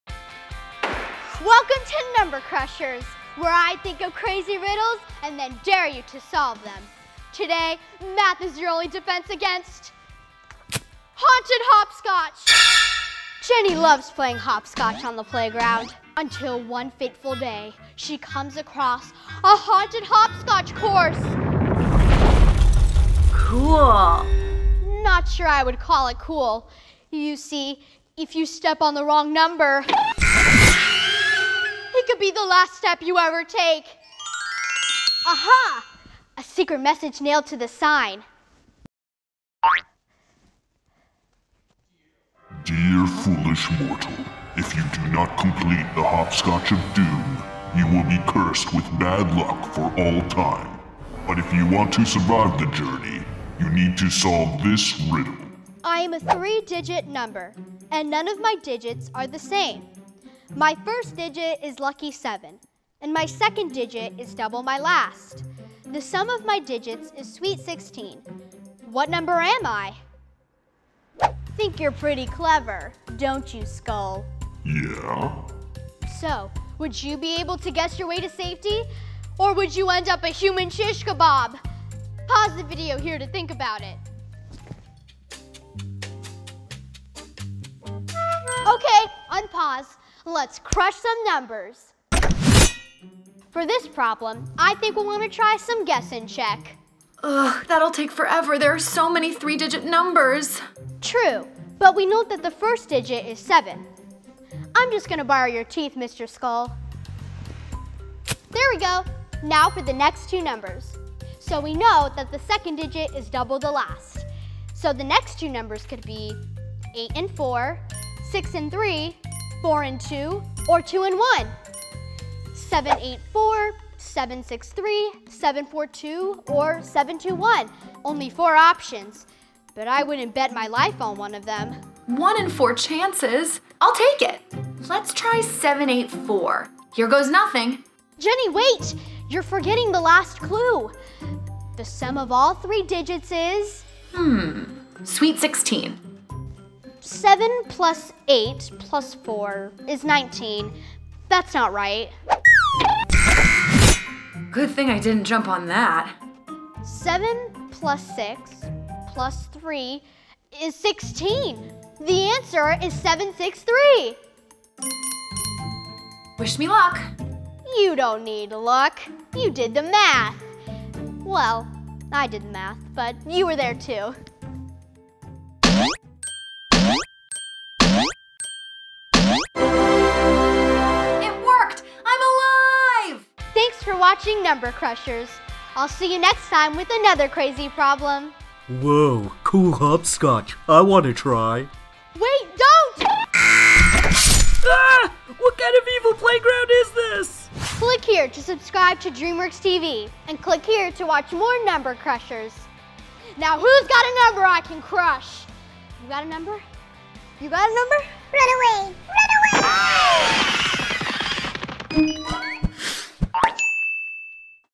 Can You Solve the Haunted Hopscotch Problem? | NUMBER CRUSHERS
Can You Solve the Haunted Hopscotch Problem? Grab your chalk and rocks because Number Wunderkid Izzy has challenged you to solve this spooky problem.

Will Jon wake up before the Zombie strikes? Will Gary sink into quicksand before his friend can save him? Take a guess or do the math to know what happens on each episode of
Number Crunchers!

→ Credits ←

Host:
Izzy Newman